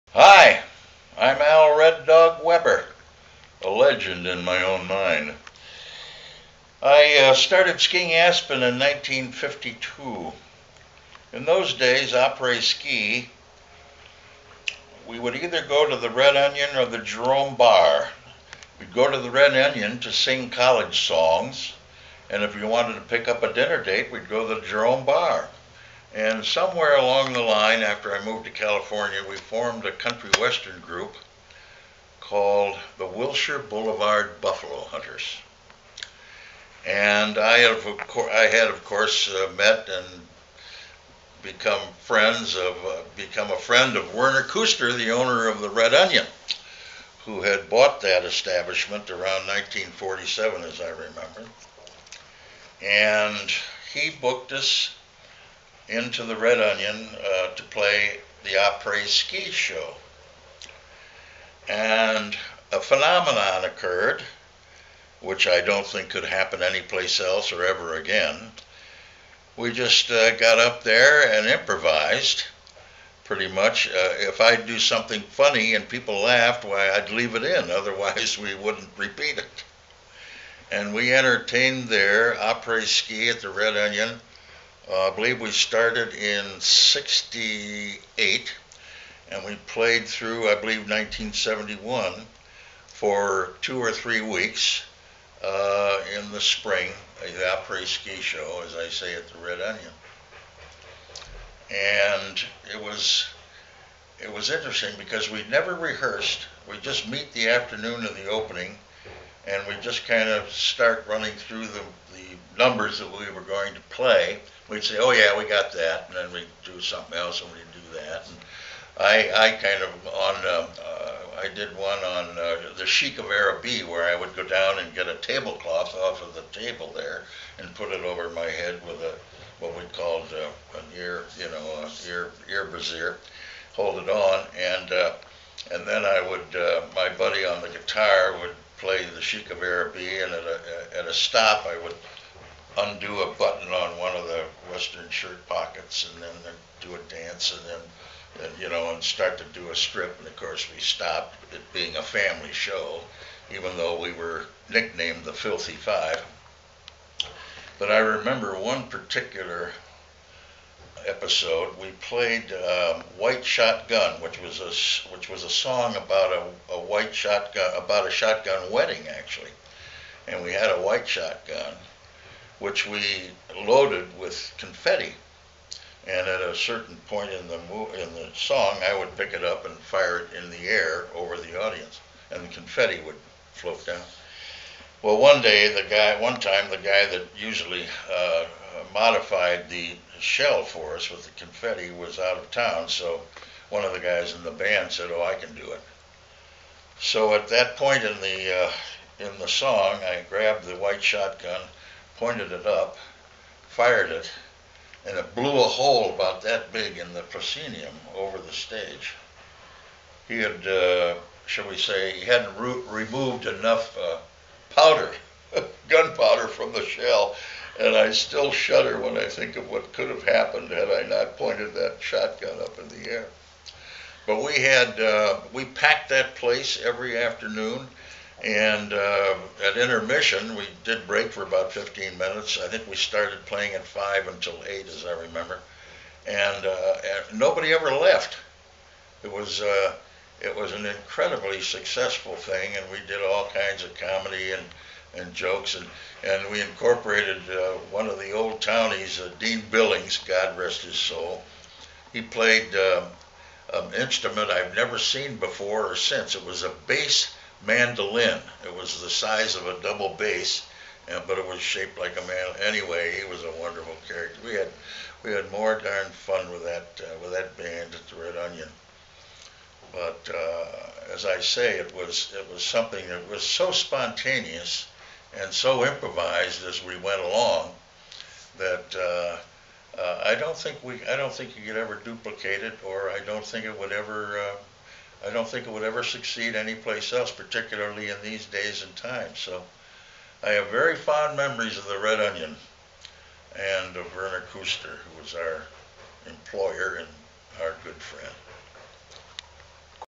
Aspen pub RedOnion Aspen pub with RedDog Weber .mov
The fabled Red Onion pub in Aspen Colorado reopens next month under new ownership, after years of
hibernation. Werner Kuster first opened it in the 50's.  Red Dog Jon Locke, and the "Buffalo Hunters" appeared there with wacko Western-style antics. Red Dog invented a percussion instrument he named the "Boom-Bass" & beat the bejesus out of it, while Jon Locke's dulcet cowboy songs charmed the crowds
that flooded the popular venue.